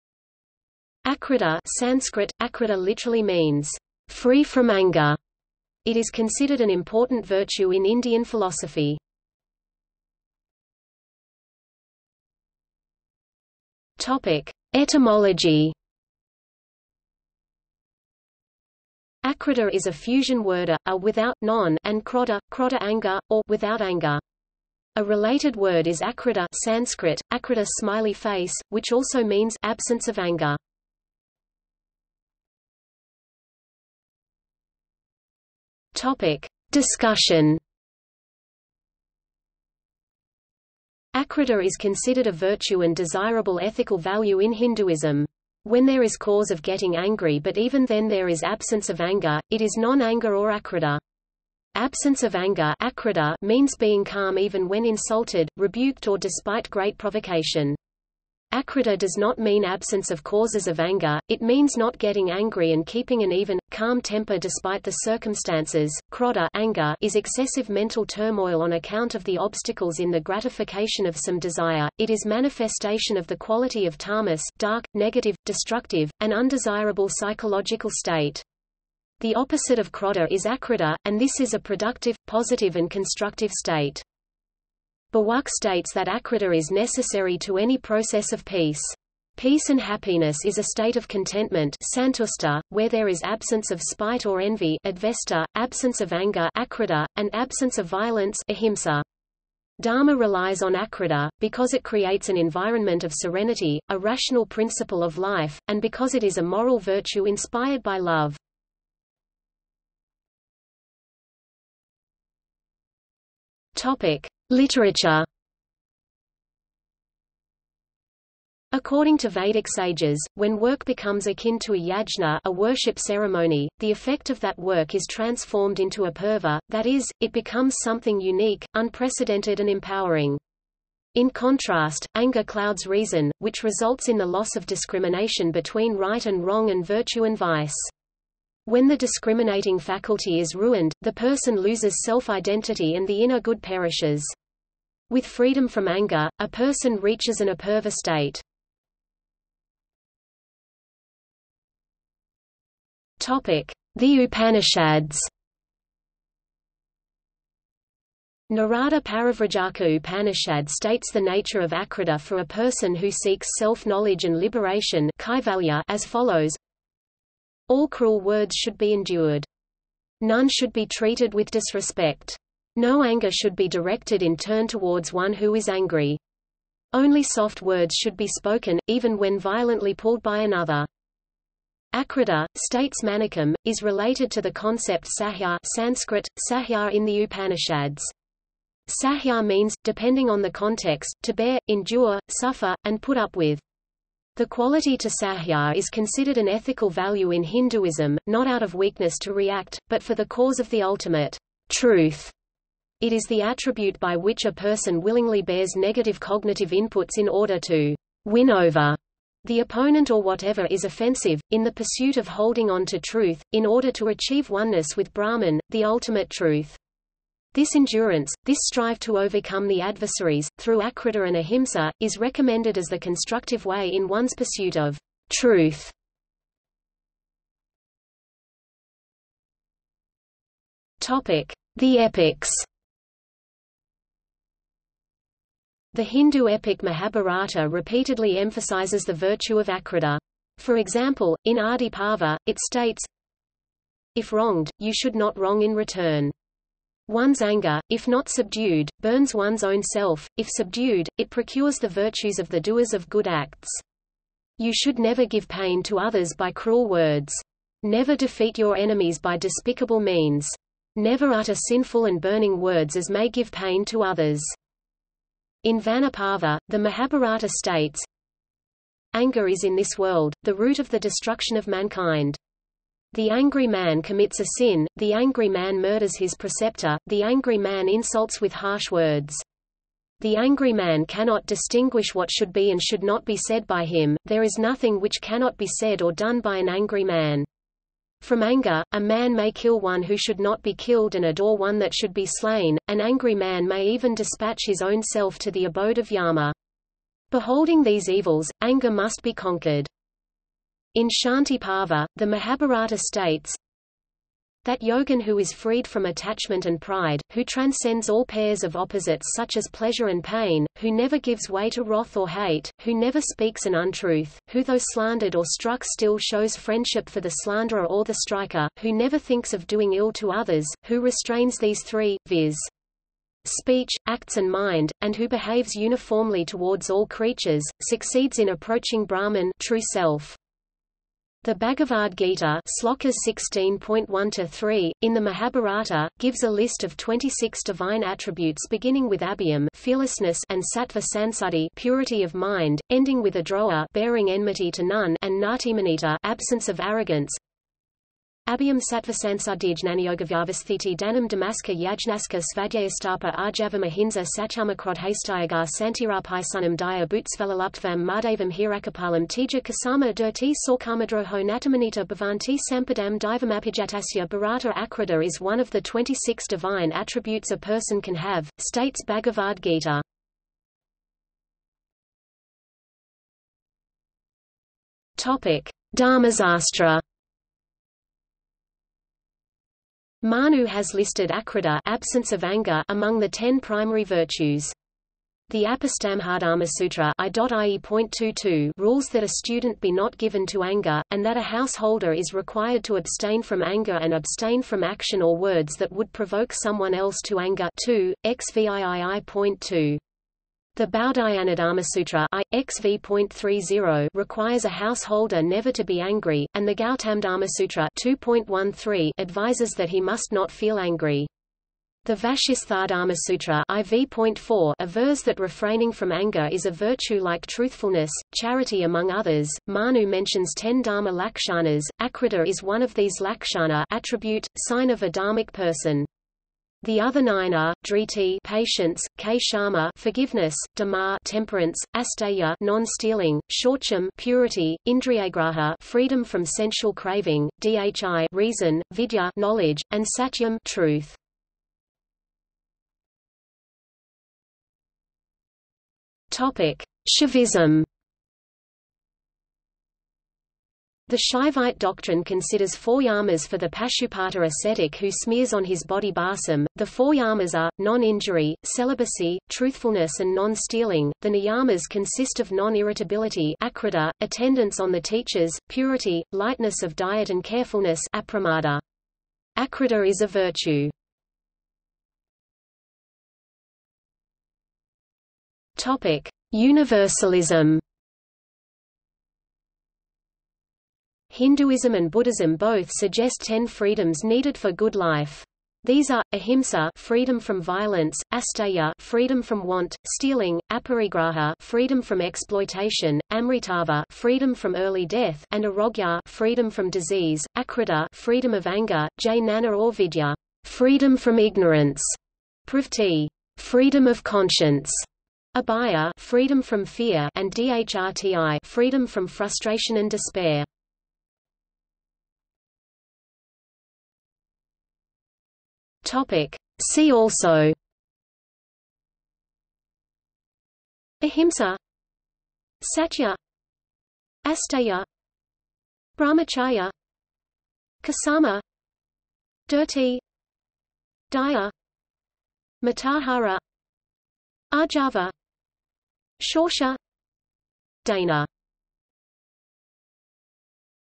Akrodha | Wikipedia audio article
00:00:10 1 Etymology
00:00:32 2 Discussion
00:01:50 3 Literature
00:02:29 3.1 The Upanishads
00:03:47 3.2 The Epics
00:06:11 3.3 Dharmasastra
00:07:23 3.4 Shaivism
00:07:52 4 Universalism
00:08:32 5 See also

- Socrates

SUMMARY
Akrodha (Sanskrit: अक्रोध) literally means "free from anger". It is considered an important virtue in Indian philosophy.